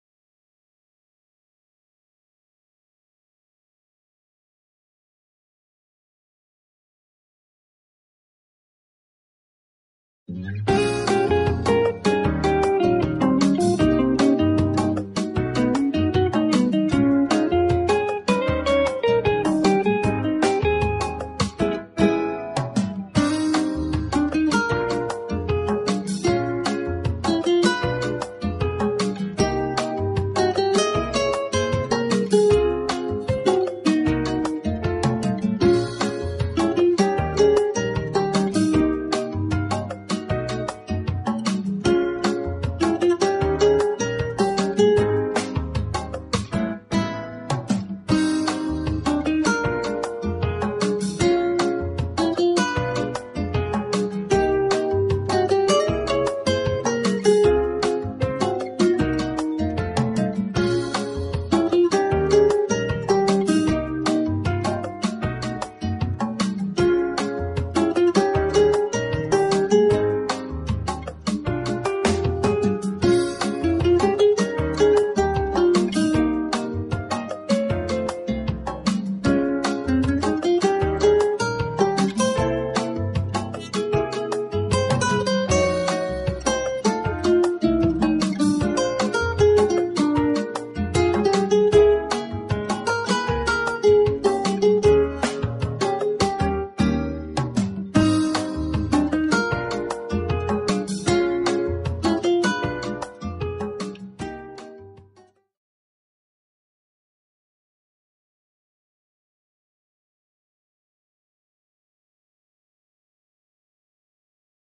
Einrichten des Kappenrahmens - Brother PR Stickserie
In diesem Video lernen Sie, wie Sie an der Brother PR Serie einen Kappenrahmen einrichten.